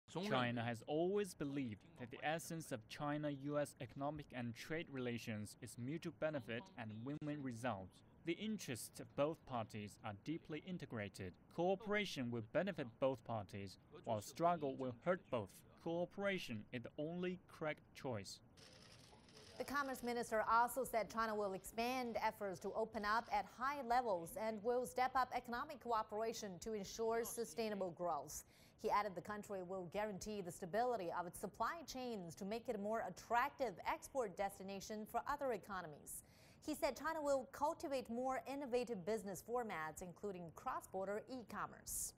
China ready to enhance dialogue with U.S. on trade
China is ready to enhance dialogue with the United States on the trade and economic fronts. Chinese Minister of Commerce Wang Wentao said Beijing seeks to bring trade relations back on track.